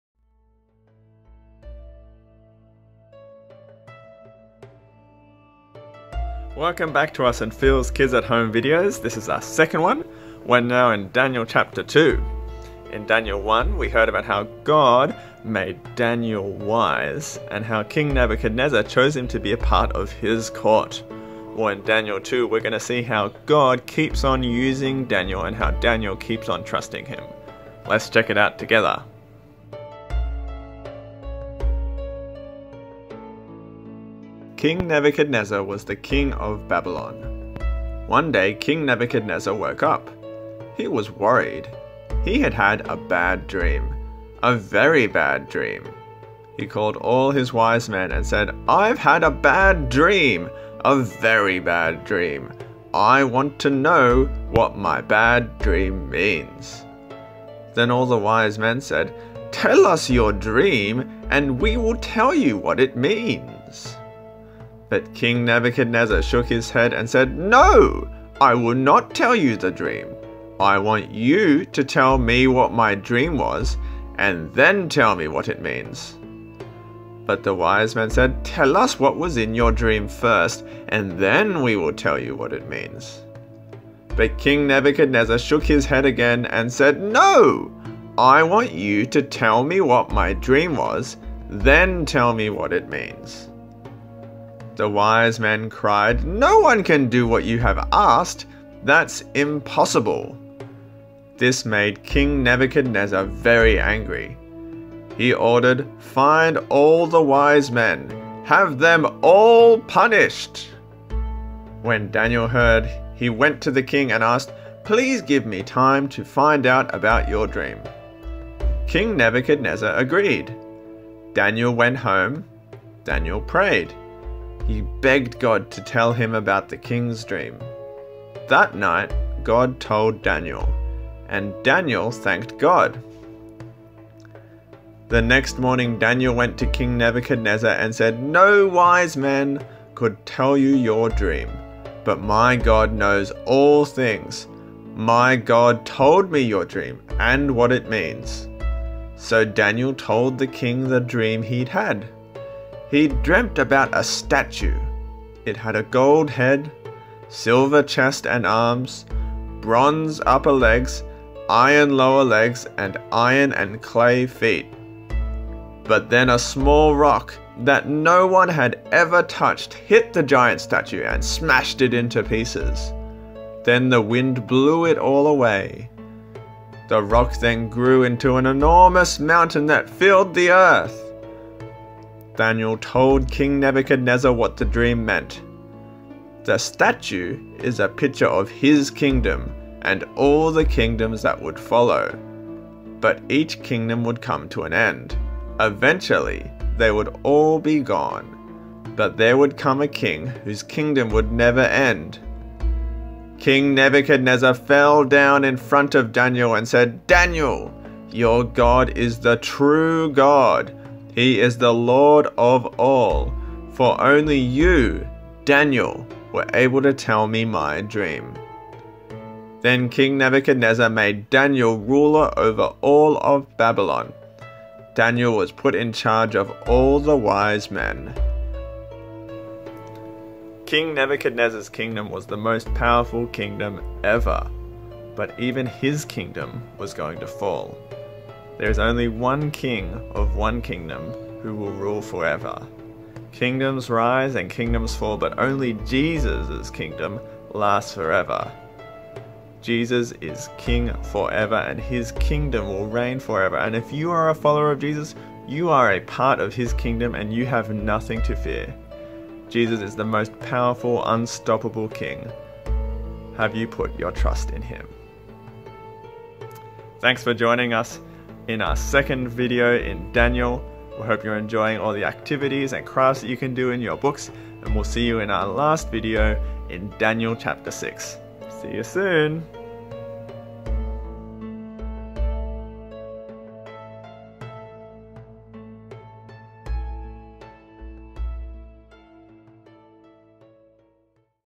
St Phils Kids @ Home | Video #2
Welcome to St Phils Kids @ Home! We're going to learn through the story of Daniel that we can trust our one true God even as life changes. Our second video is in Daniel Chapter 2

Script from Kidswise Daniel Unit by Sandy Galea
Illustrations by Jo Lum

If you have not received a St Phils Kids @ Home Activity Book fill in a St Phils online connect form to request one